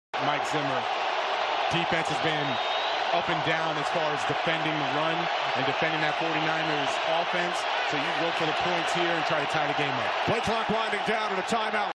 Vikings Kirk Cousins standing behind His Right Guard On 4th & Goal
Kirk Cousins standing behind His Right Guard On 4th & Goal then Vikings called timeout because he is lined up behind the wrong lineman
Something is always wrong with Vikings